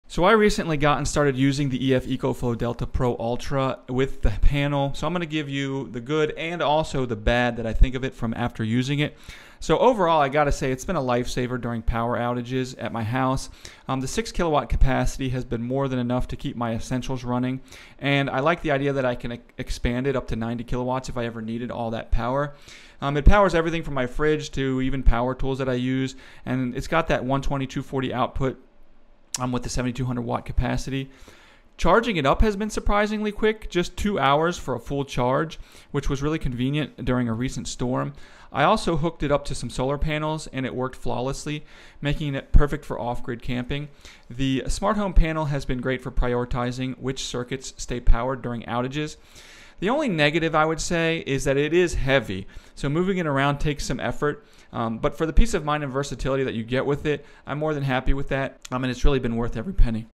Whole Home Battery Backup for Under 10k With or Without Solar
Quick look at the EF ECOFLOW DELTA Pro Ultra with Smart Home Panel, 6000Wh Power Station, 120/240V 7200W AC Output, Lifepo4 Home Battery Backup Expandable to 90kWh, 2H to Full Charge, Solar Generator for Home Use, RV.